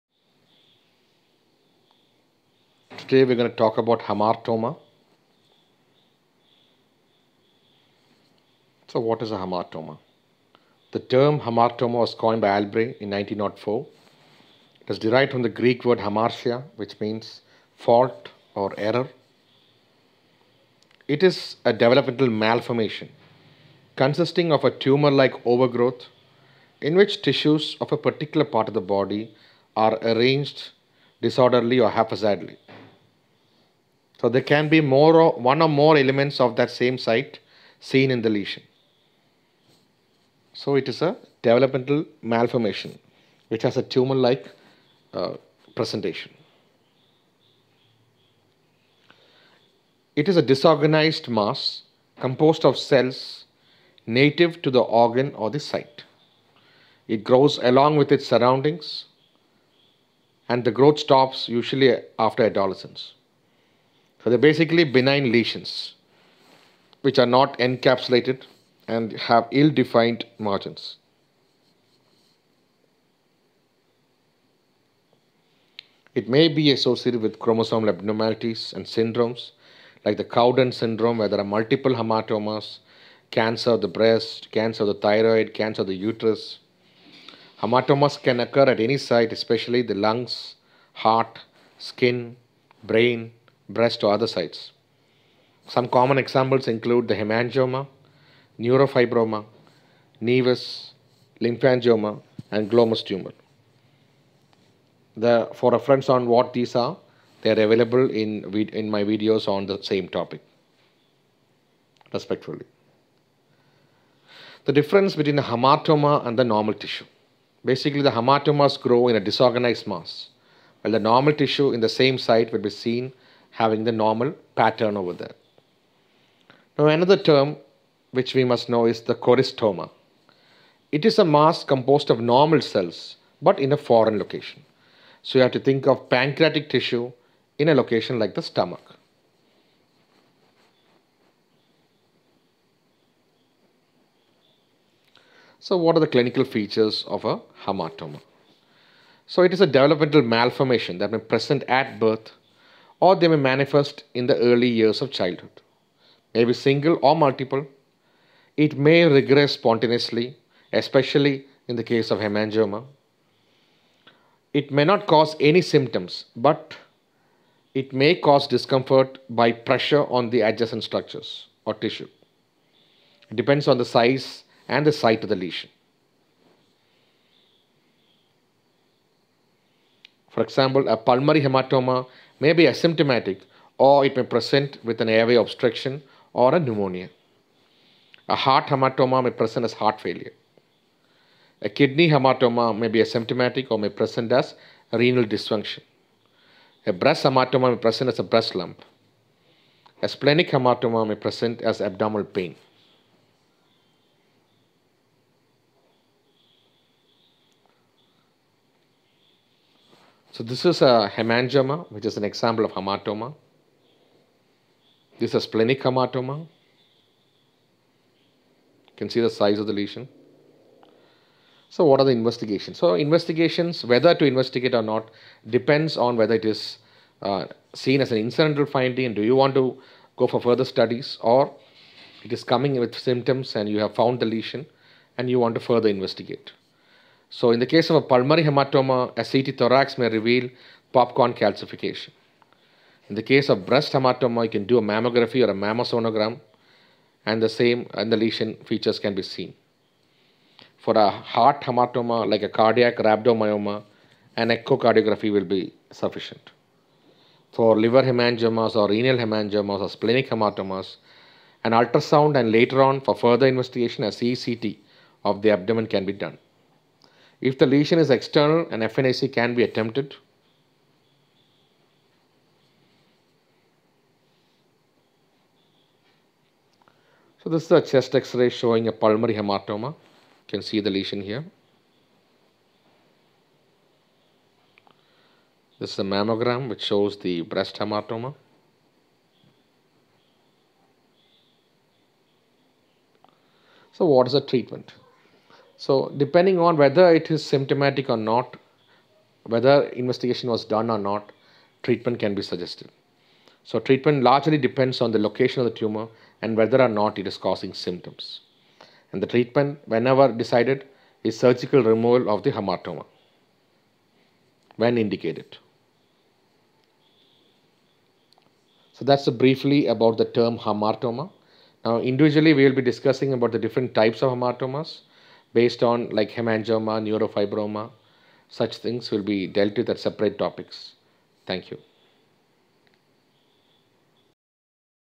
Hamartoma
Brief description of term hamartoma, it's clinical presentation and treatment for purpose of understanding of undergraduate students (MBBS, BDS?)